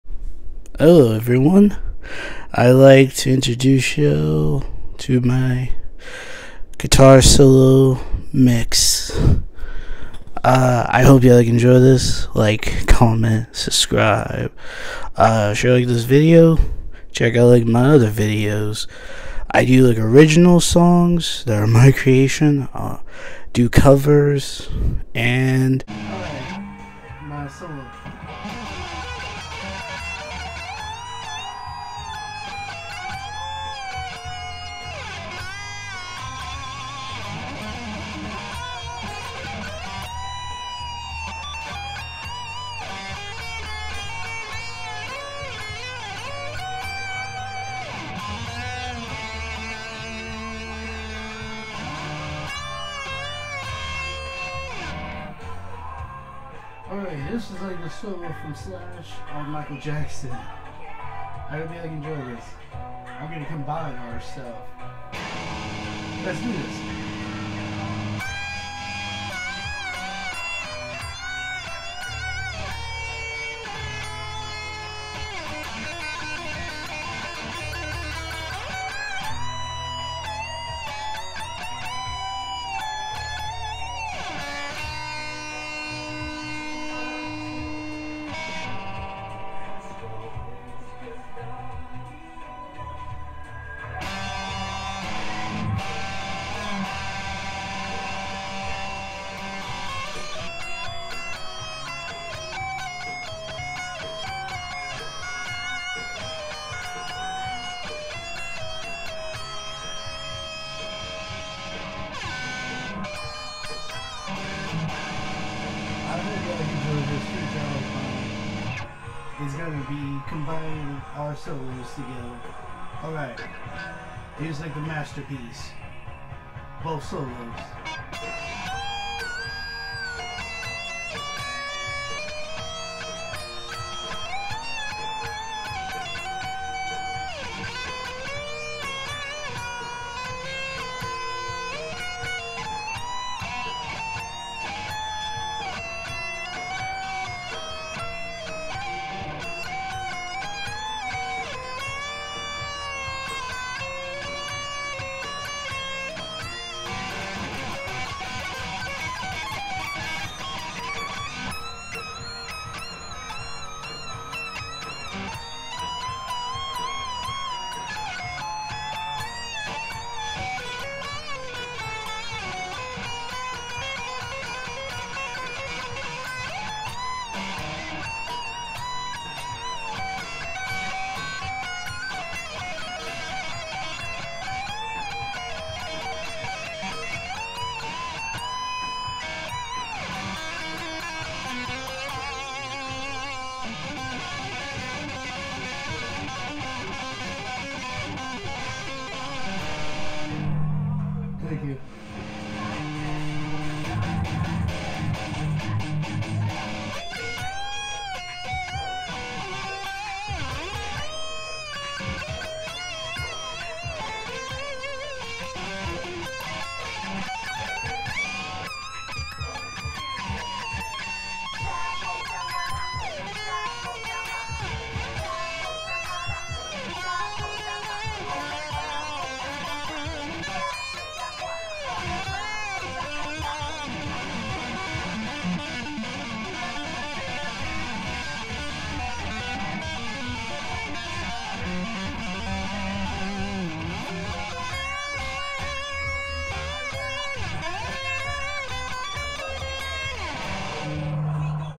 Guitar solo mix by prince jc
This is something I created of my two favorite guitarist solo with a kpop solo with original ideas. It has a great thing. Like gun n roses slash is one of my favorites. This guitar mix is fun and creative. I hope everyone enjoys this.